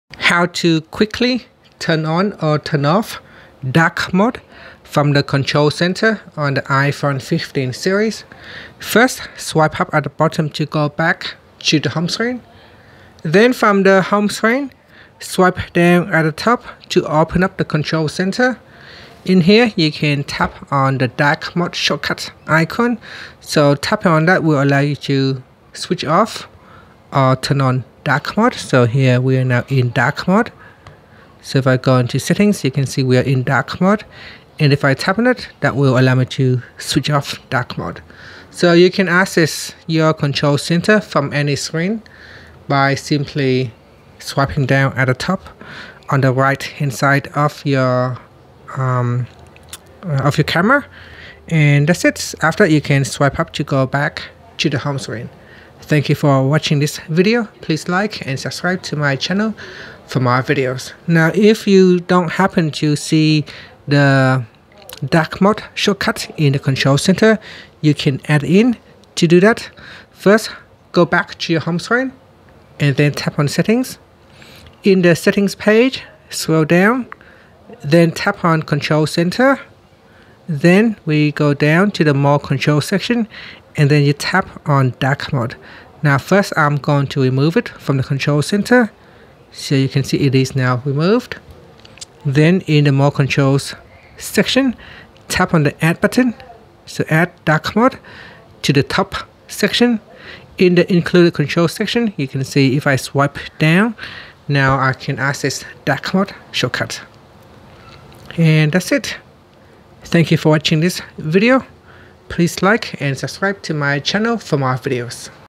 iPhone 15/15 Pro Max: How to Quickly Turn On/Off Dark Mode From the Control Center
Learn how you can quickly turn On/Off Dark Mode from the iPhone 15 control center.

Say goodbye to fumbling through settings! This video reveals the lightning-fast way to toggle Dark Mode on your brand new iPhone 15, 15 Pro, 15 Pro Max, and 15 Plus running iOS 17. Learn how to instantly switch between light and dark themes from the convenient Control Center, saving you precious time and energy. Whether you're a night owl embracing the dark side or a sun seeker craving brightness, this hack is a must-have!

Here's what you'll learn:

- One-tap Dark Mode activation from the Control Center
- Customizing the Control Center for instant access